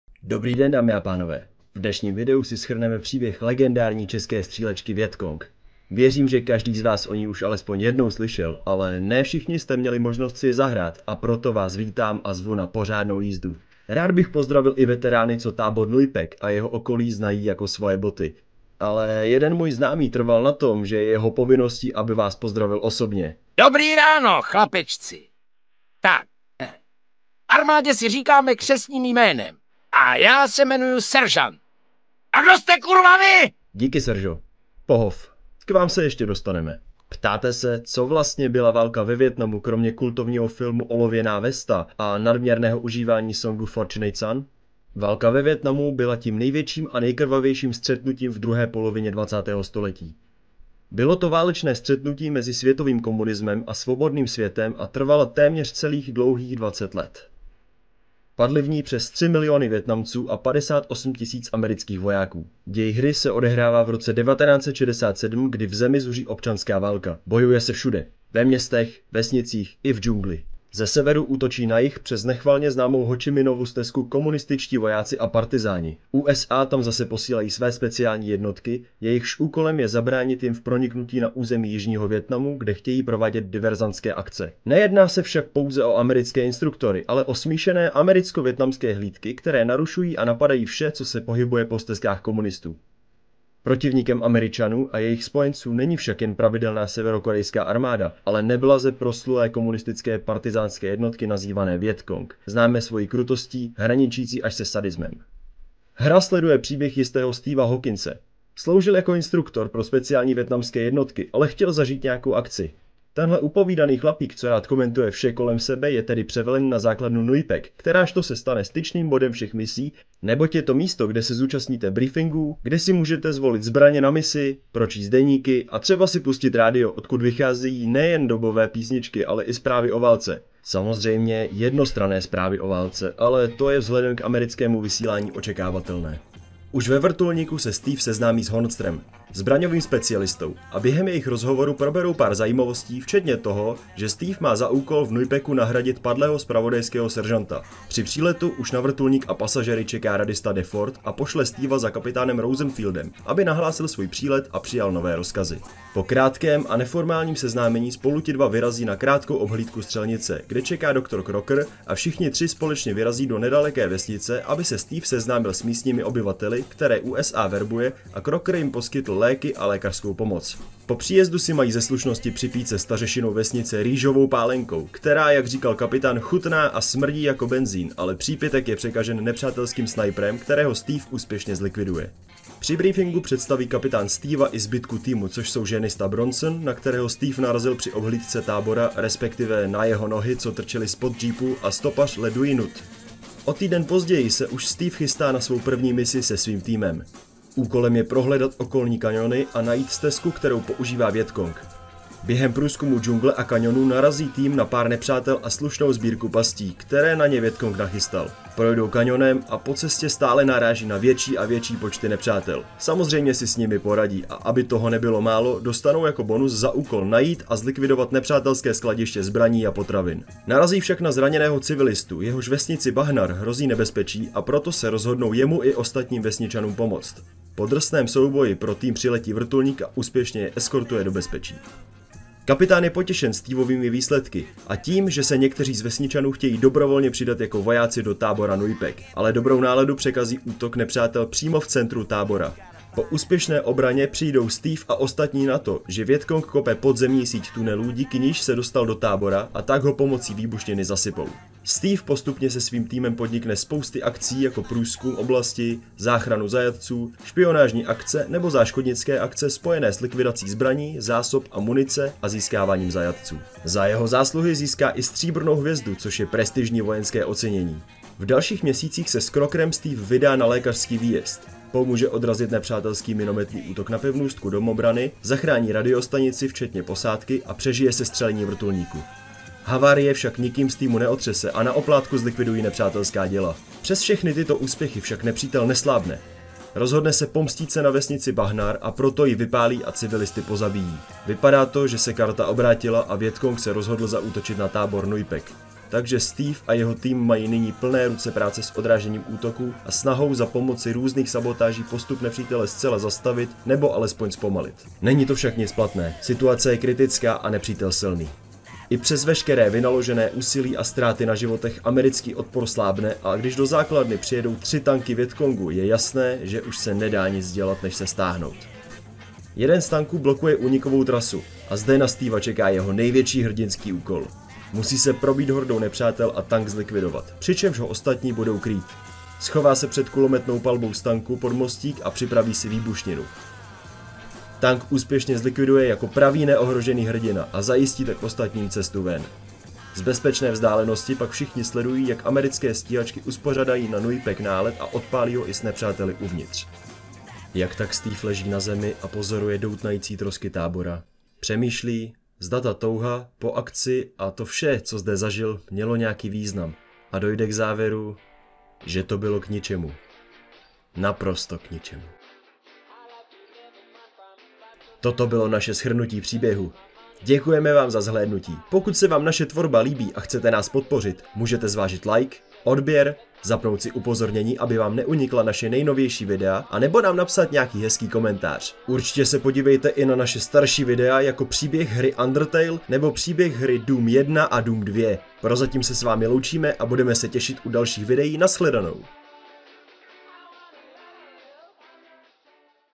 Vietcong CZ PŘÍBĚH/LORE | Bratři z Dungeonu
Kdo by neznal legendární českou střílečku Vietcong? Říká se, že je hodně těžká, ale věděli jste, že to, co tuto hru dělá tak skvělou je její příběh? Posaďte se a zaposlouchejte se do našeho videa!

Dabing: ScripThor
Střih: MegaFunMan

Zdroje: Dodáme na požádání